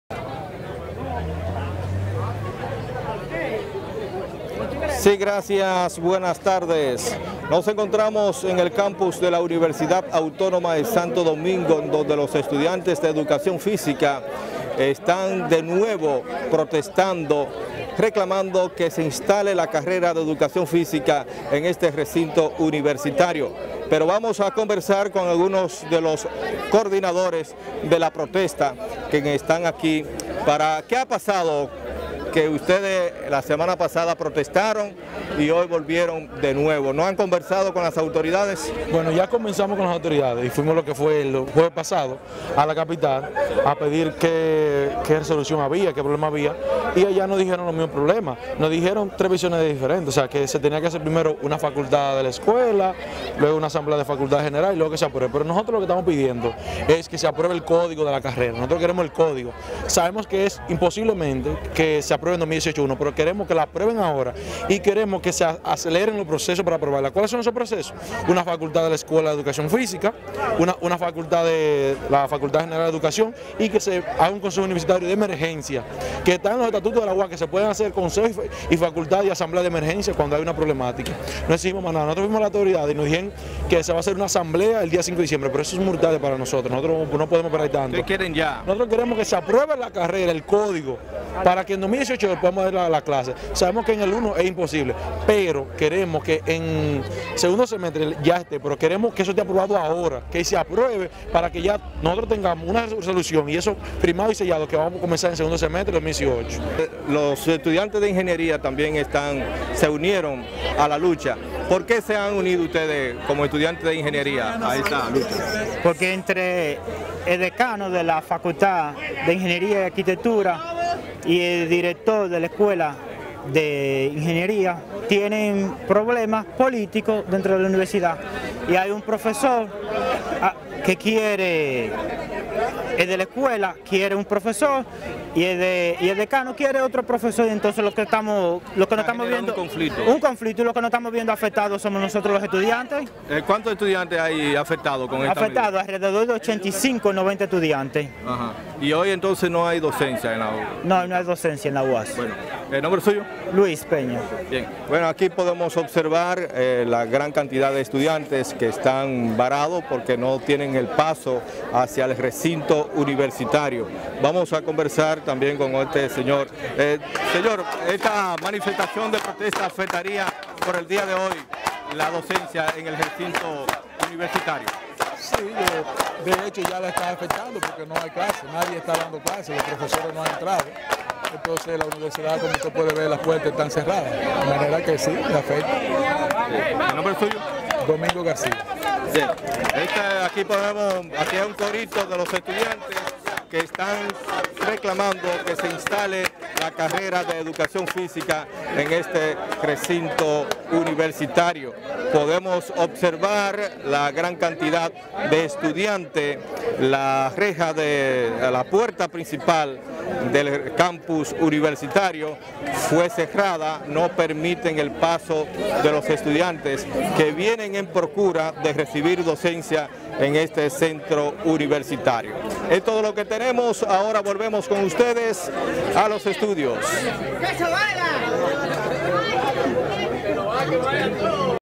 Estudiantes del recinto de la UASD en Santiago protestan
Protesta UASD    /22/11-2017
Rafael Martínez/Santiago
Duración  / 1:33

LEAD…

Las clases en el Recinto-Santiago de la Universidad Autónoma de Santo Domingo fueron suspendidas este miércoles por estudiantes en reclamo de la incorporación al pensum universitario la carrera de educación física.